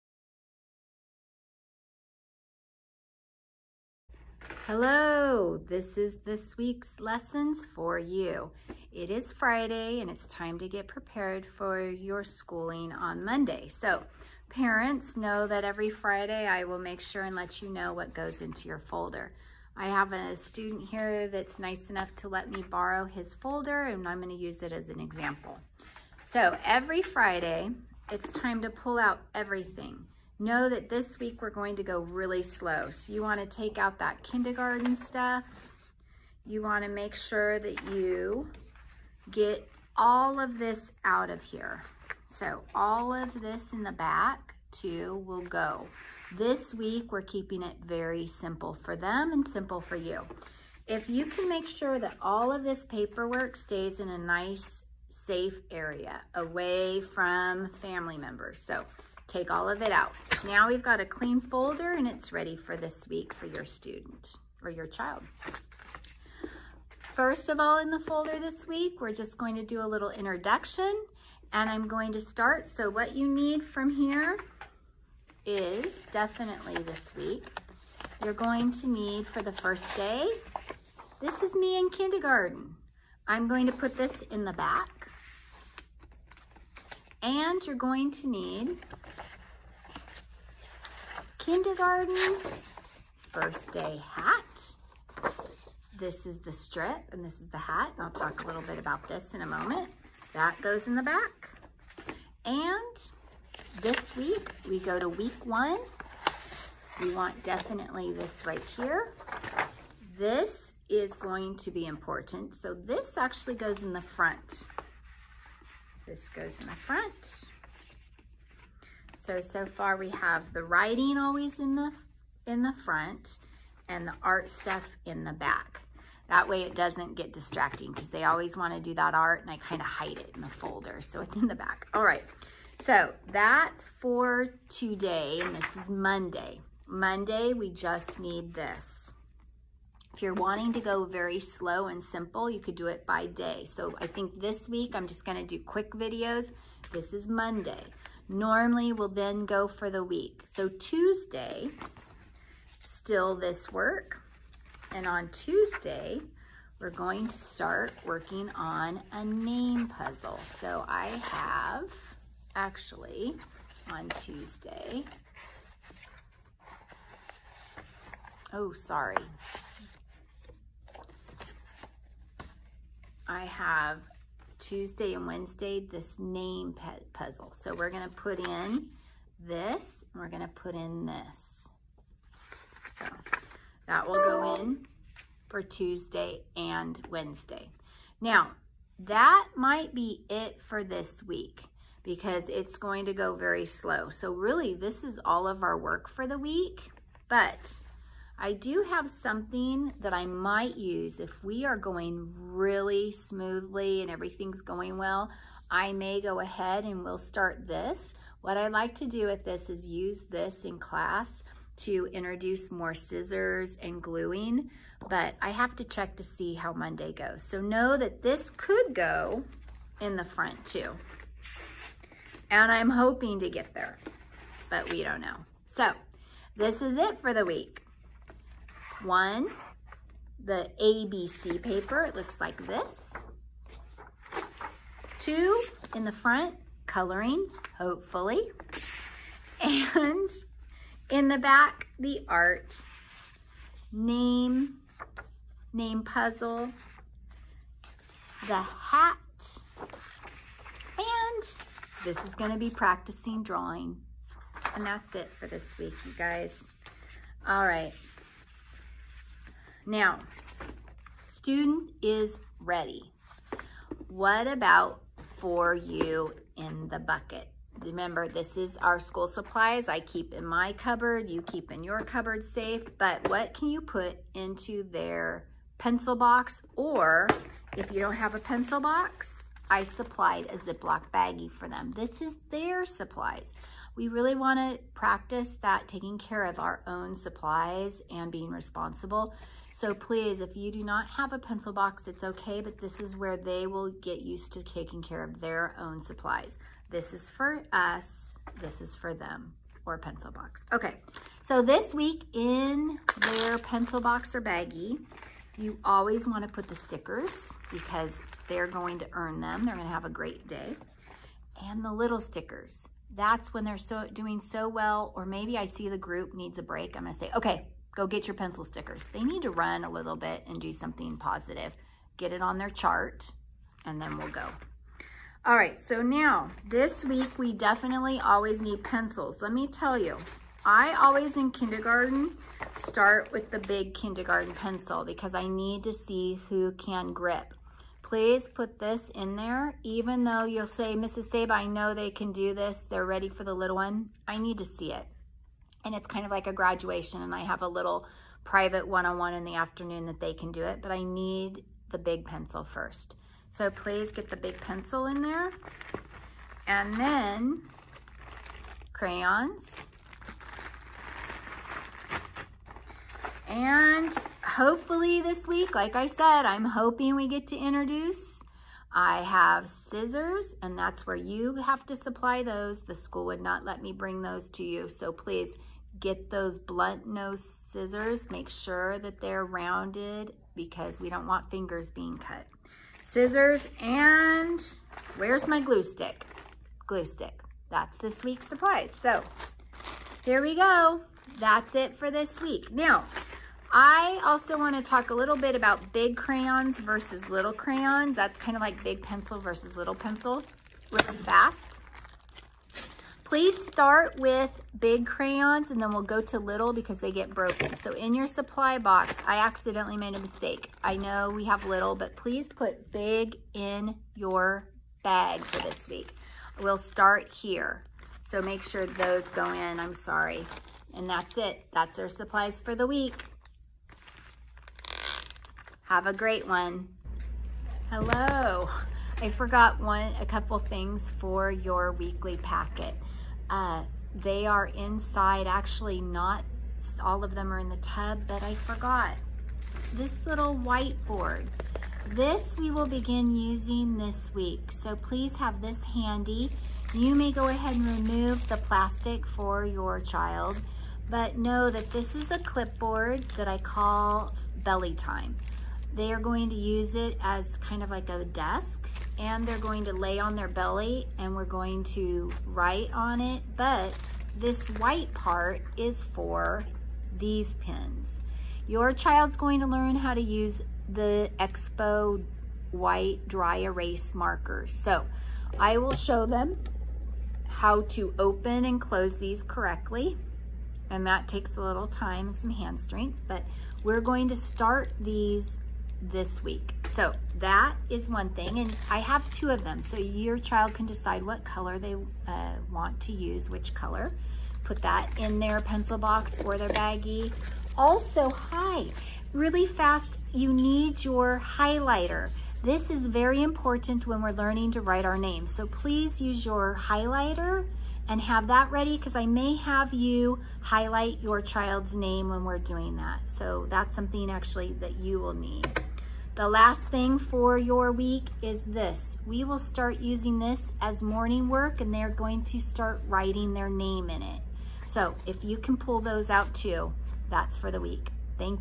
Parent Plans Week 1: 8/17 - 8/21
Items for pencil box / zip lock bag:
expo marker
pom pom (eraser for the board- I forgot this in the video)
highlighter
big crayons
big pencil
glue stick
white glue (parent at home will help use glue on black paper, tracing name)
 stickers

Items for folder:
first day hat
kindergarten picture paper
ABC week 1 papers
name highlighter puzzle
black name/color construction squares
color book
white board/clipboard